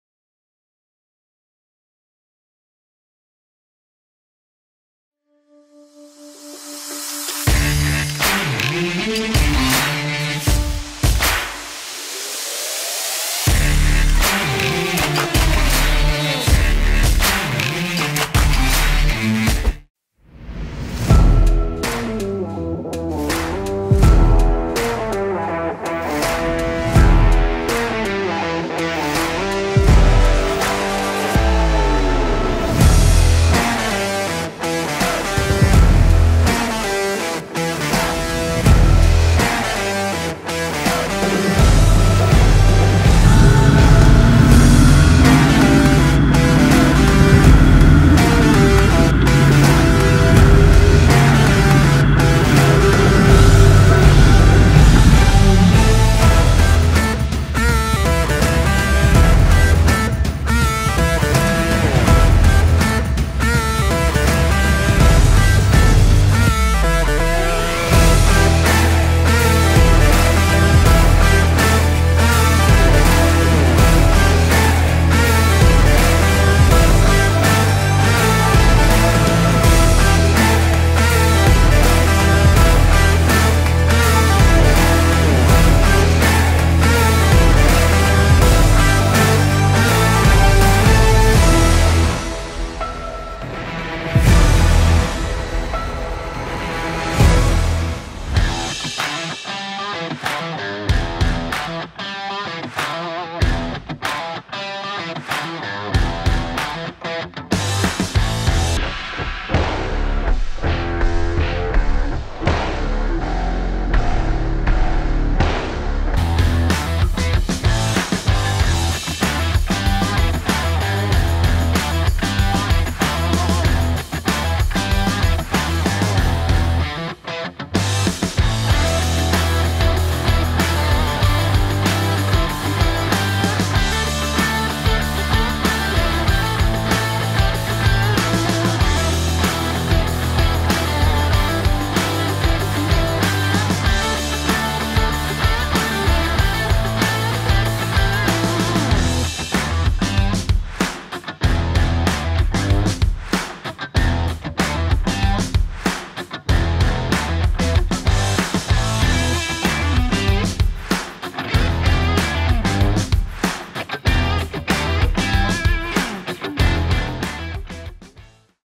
DEV LASTİK DAMACANAYI YOK ETTİ! | EZ GEÇ #7
Ez geç serisinde bu hafta 55 cm'lik bir tekerlekli fil ve su dolu damacana eziyoruz. Bu tarz rahatlatıcı ezme videolarının devamın için videoyu beğenmeyi unutmayın!